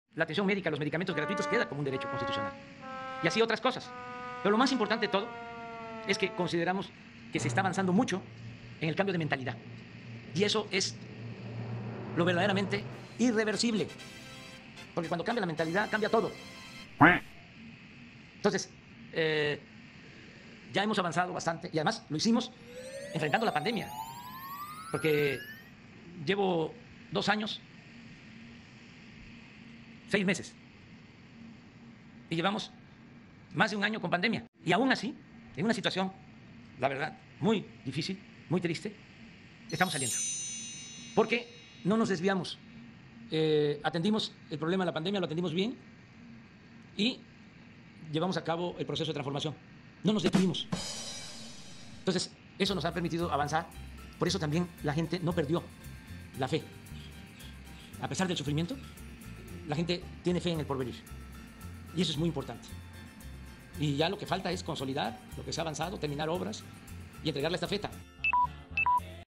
No nos desviamos atendimos la pandemia y no detuvimos el proceso de transformación 🦆
Consideramos que se está avanzando mucho en el cambio de mentalidad, no nos desviamos atendimos la pandemia y no detuvimos el proceso de transformación 🦆